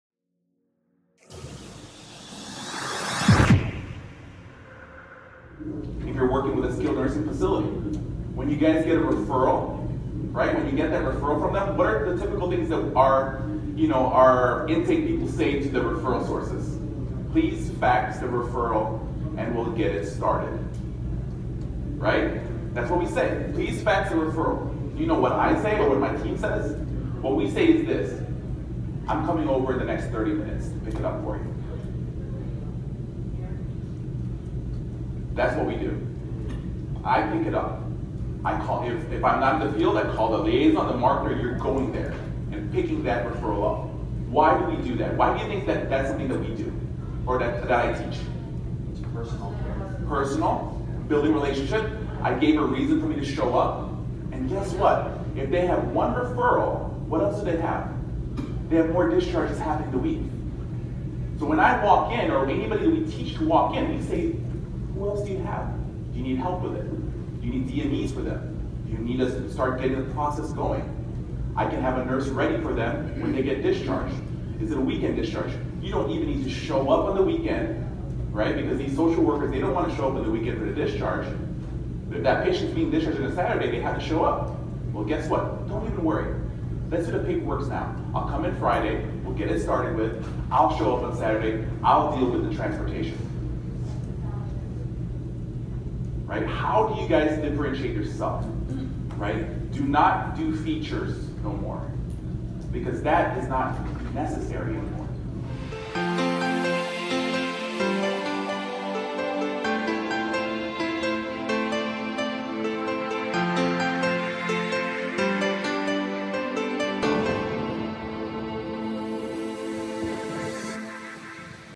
Correct and Wrong Way to Get a Referral - Home Health Marketing
Here is the correct way to recieve a referral and why this will make you stand out from other agencies.